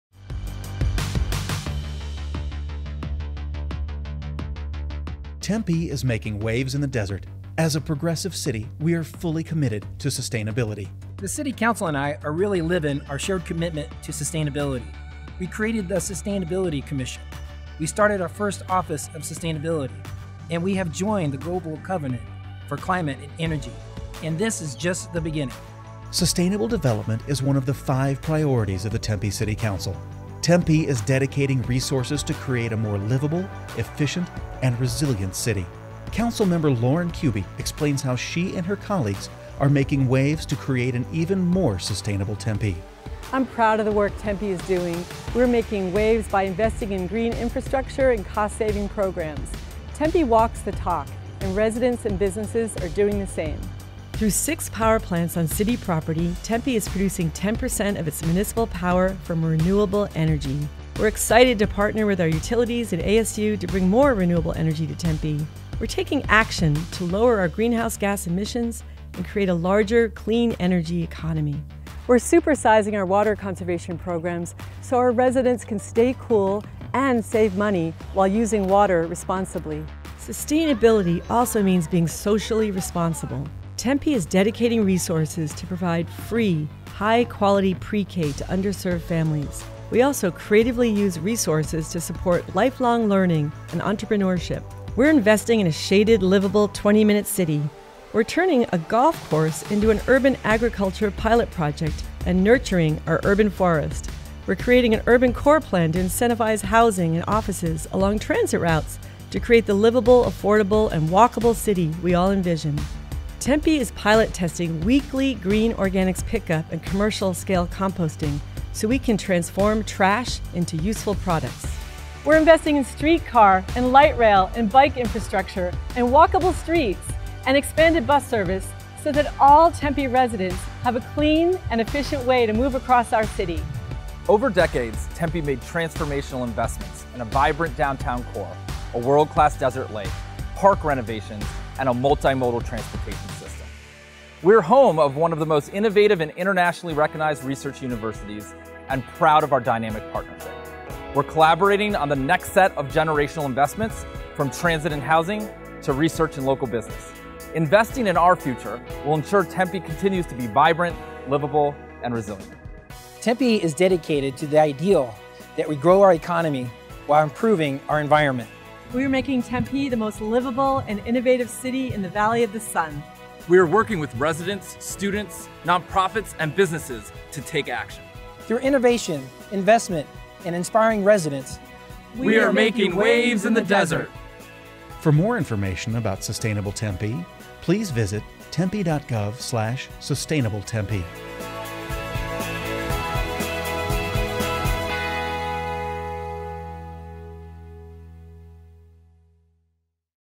Sustainable Tempe
Councilmember Lauren Kuby highlights the many sustainable programs that have been implemented in the City of Tempe